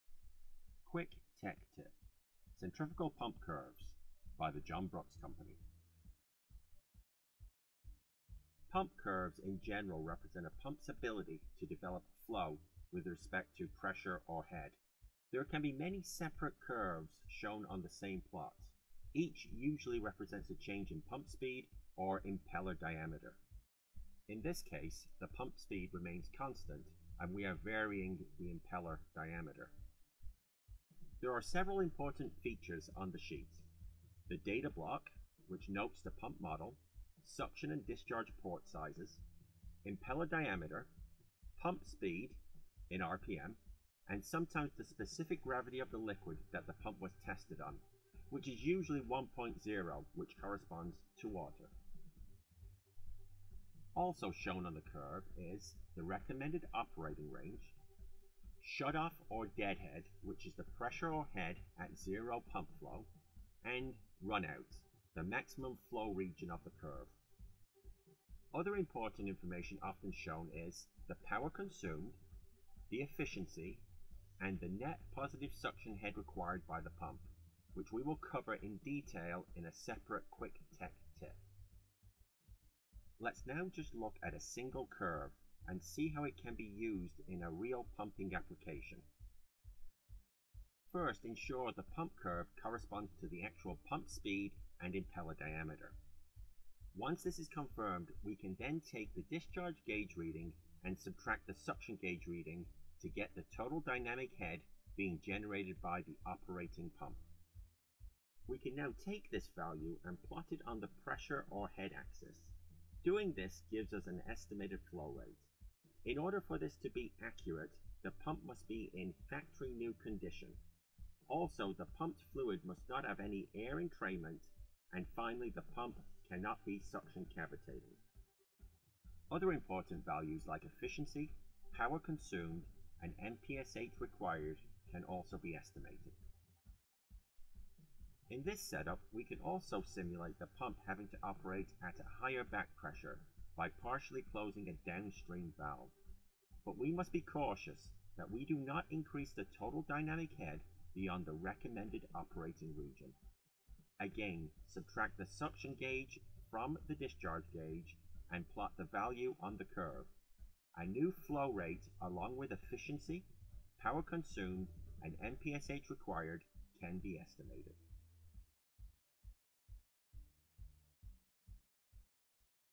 Centrifugal Pump Curves by John Brooks Company
Pump curves, in general, represent a pump's ability to develop flow concerning pressure or head. This Quick Tech Tip by our application expert Stefan Fediw examines the different features of pump curves, and how a curve is used in a pumping application.